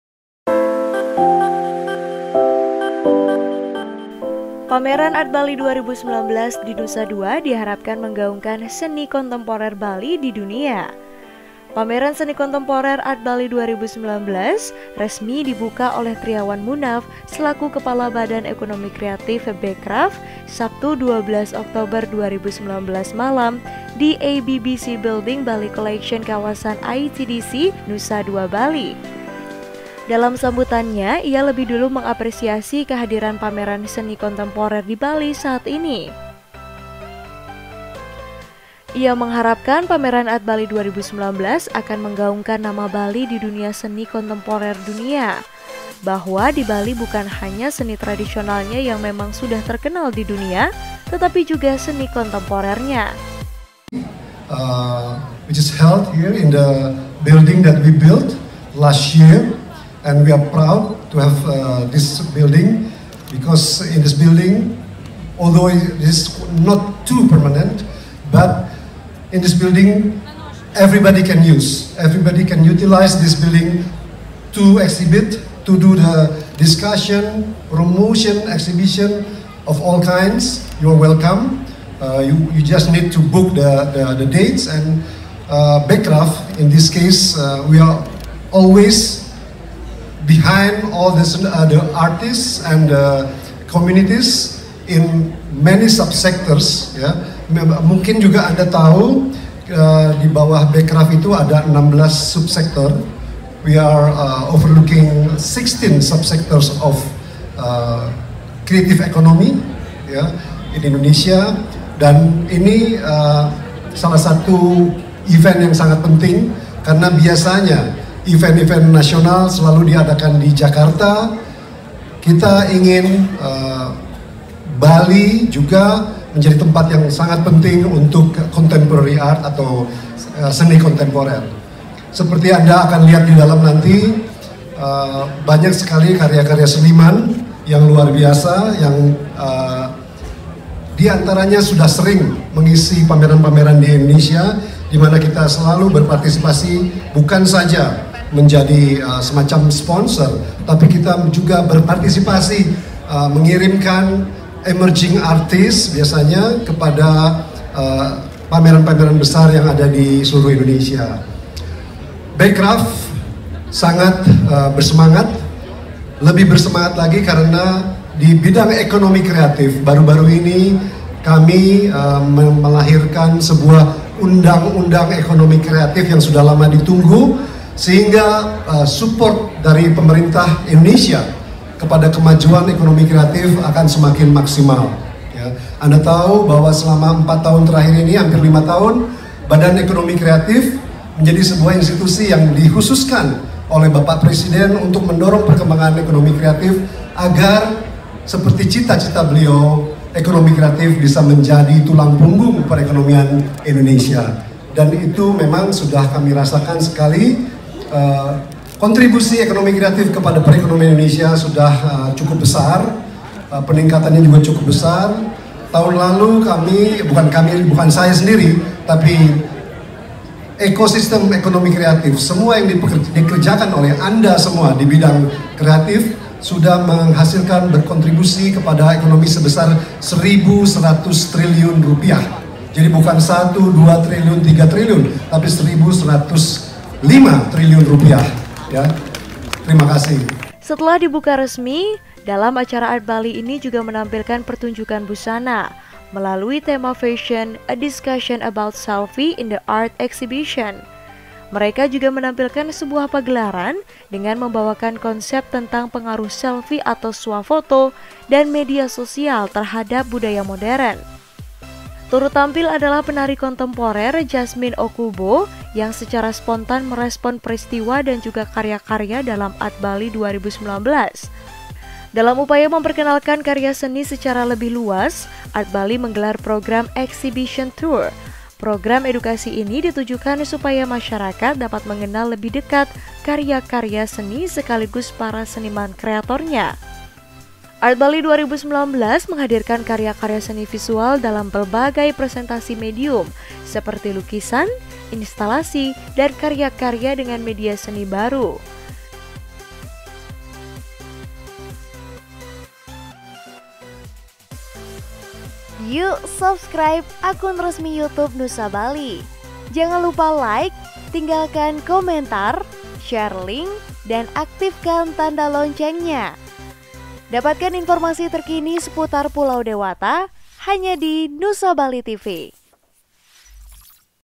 Art Bali 2019 : Contemporary Art Exhibition
Pameran seni kontemporer Art Bali 2019 resmi dibuka oleh Triawan Munaf selaku Kepala Badan Ekonomi Kreatif (Bekraf), Sabtu (12/10/2019) malam di AB-BC Building, Bali Collection, Kawasan ITDC, Nusa Dua, Bali yang digelar hingga 13 Januari 2020 mendatang.

Reporter : Zakky
Videographer : Anita DS